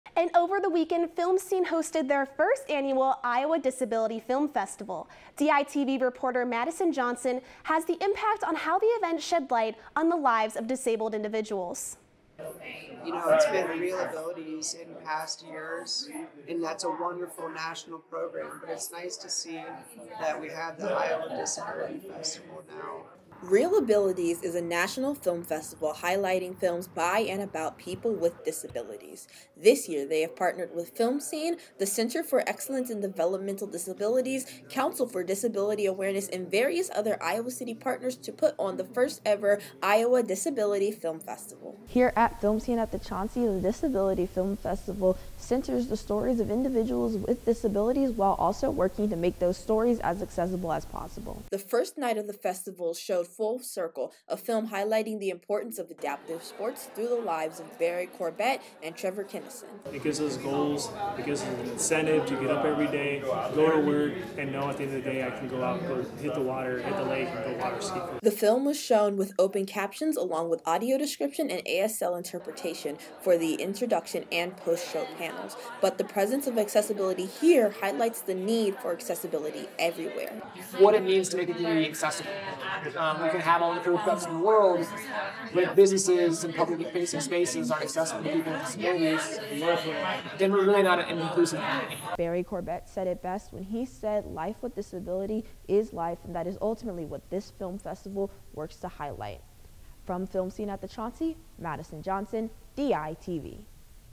DITV: Iowa Disability Film Festival
Last Weekend Filmscene hosted the first annual Iowa Disability Film Festival which highlighted the lives and voices of disabled individuals